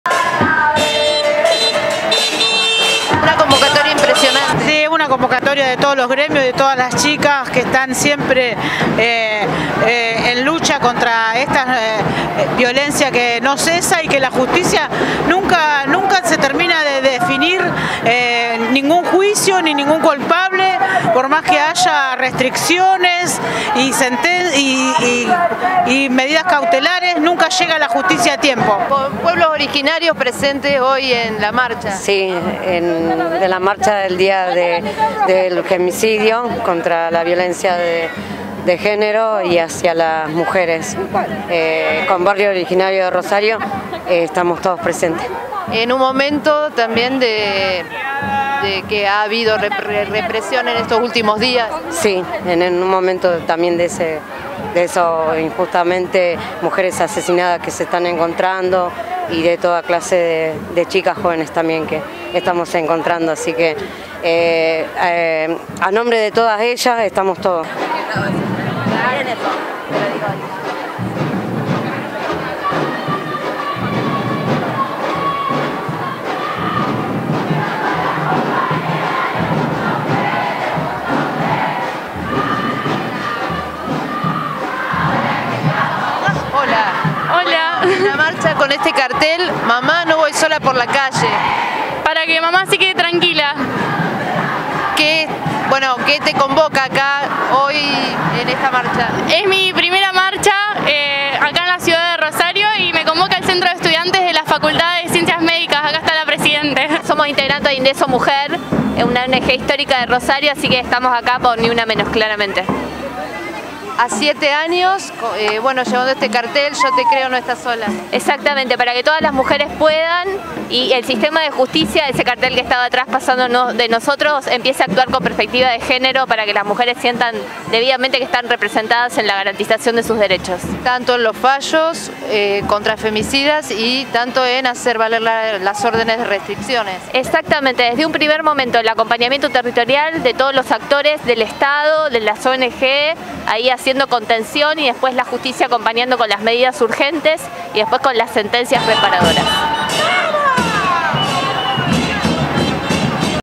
Miles de personas marcharon por la ciudad en reclamo de “Ni una Menos”
Unas 30 cuadras de gente se movilizaron en Rosario bajo la consigna "El Estado es responsable". Como cada 3 de junio, se renueva el pedido por mayor equidad y contra la violencia de género.